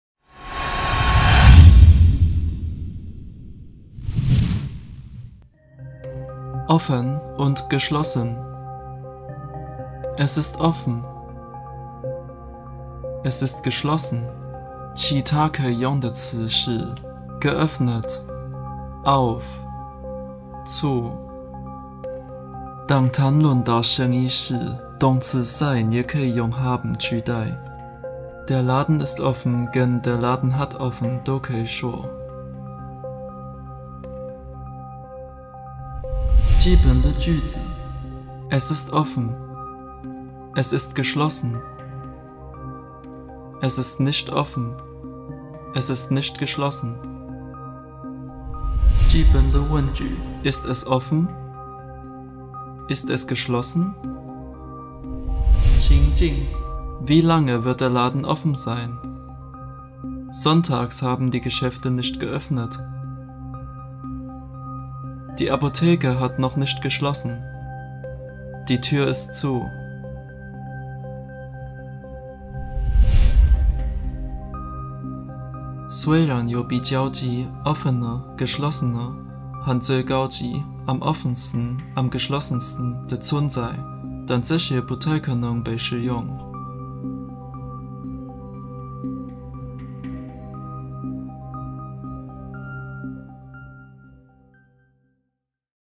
開的/營業 & 關的/休業 [offen & geschlossen] － 德文學到死
德文形容詞 [offen & geschlossen | 開的/營業 & 關的/休業] 的摘要！

您的英文好嗎？英文的文法常常和德文的文法相近，來觀看這個詞彙的英文版影片會讓您更了解！

影片也包含：
「它是開的/現在營業 / 它是關的/現在休業」 的德文怎麼說
「這家店是營業的」 的德文怎麼說
「它不是開的 / 它不是關的」 的德文怎麼說
「它是 (那麼 /太) 開的 / 關的嗎」 的德文怎麼問

範例句子：
(1) 這家店營業到幾點？ (2) 這些店禮拜天都沒有營業。
(3) 藥局還沒休業。 (4) 門是關的。

Lern Deutsch, Deutsch lernen, 學德文，學德語
[offen, geschlossen, geöffnet, auf, zu, offener, offenste, geschlossener, geschlossenste]